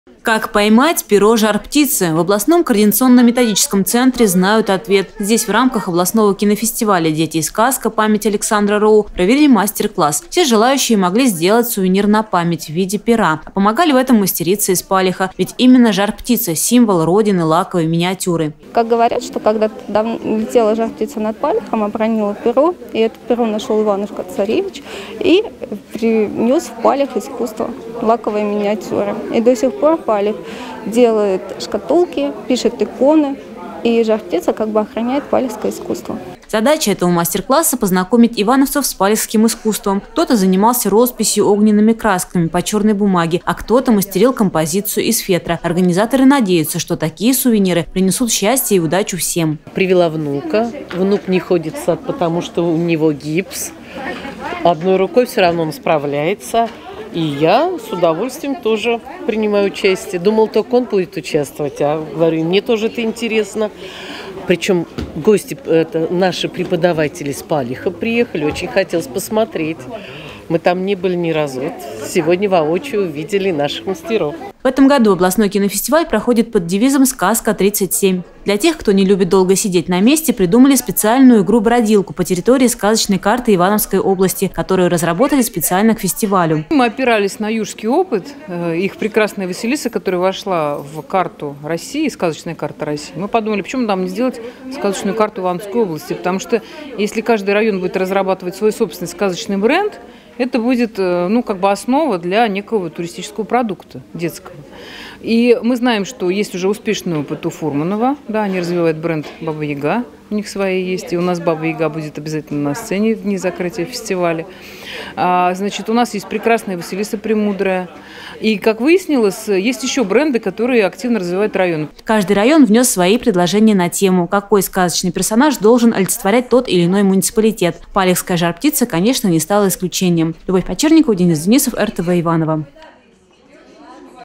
Волшебное перо «Жар-птицы»
Как поймать перо Жар-Птицы? В областном центре культуры и творчества знают ответ. Здесь в рамках областного кинофестиваля «Дети и сказка. Памяти Александра Роу» провели мастер-класс.